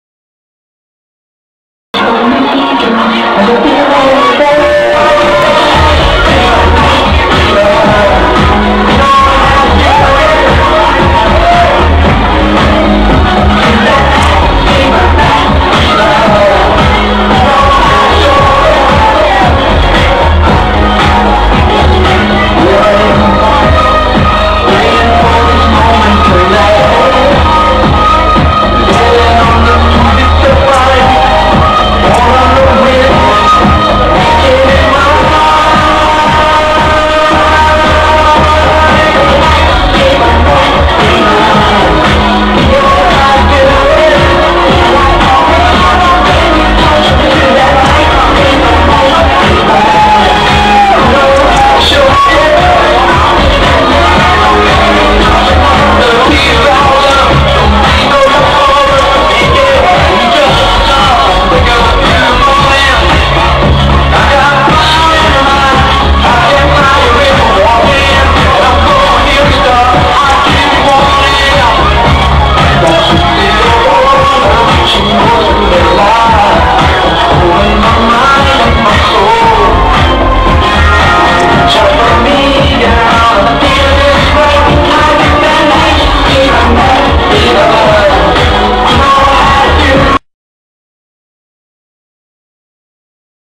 Night fever at the Flying Handbag Blackpool
Disco Kids reunion in Blackpool a few years back.